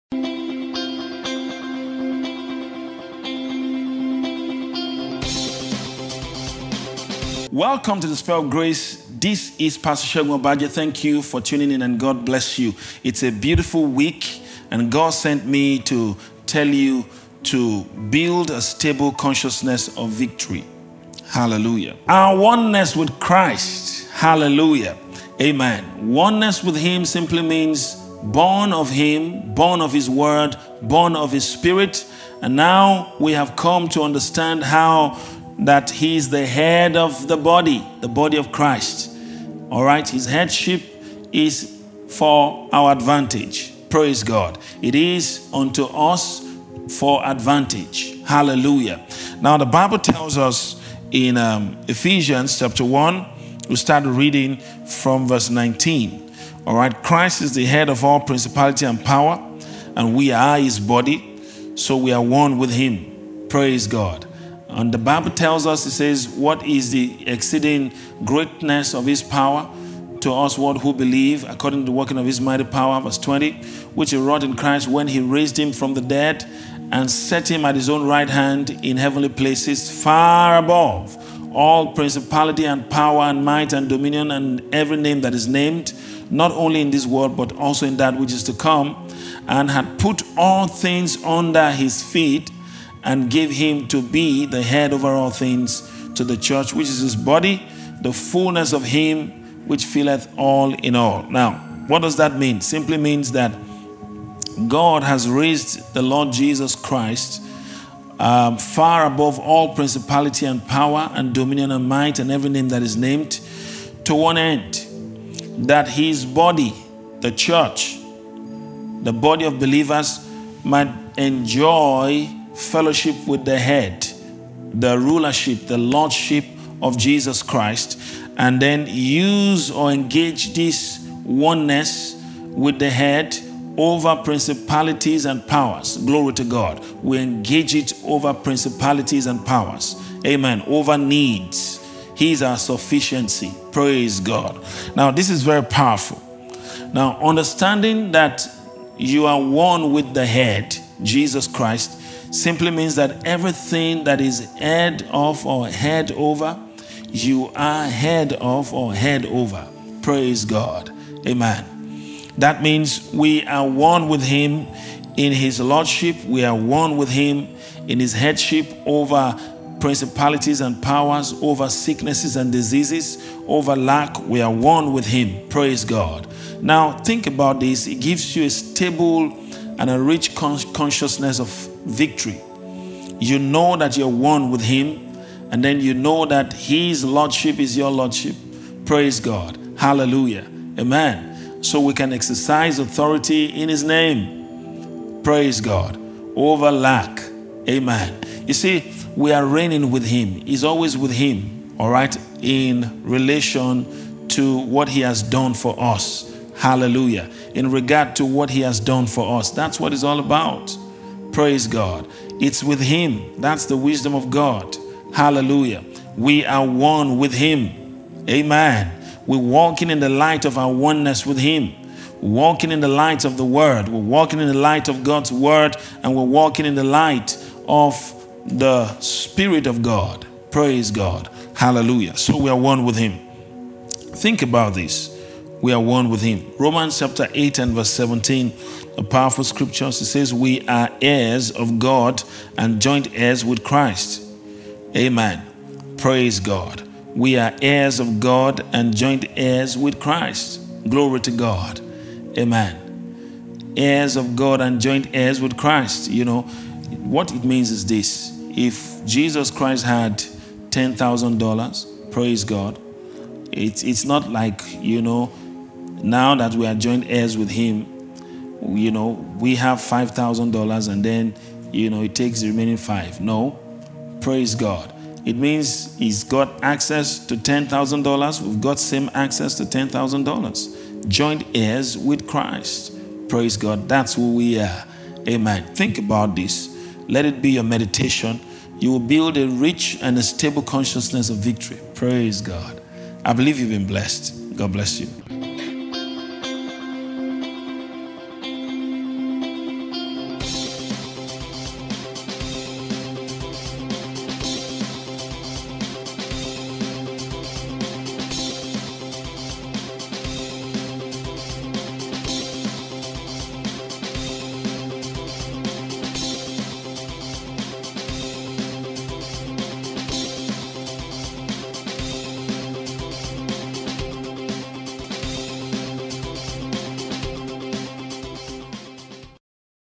May 24: Our Oneness With Christ By Pastor Segun Obadje (E1185)
Christ is the head of the church, and we are His body, so we are one with Christ, we have fellowship with Him and we use this fellowship over principalities and powers. Christ is Lord over all, lord over sickness and diseases, lord over health, over money, over demon spirit and His lordship is our lordship, so we are lord over demon spirit as well, hallelujah.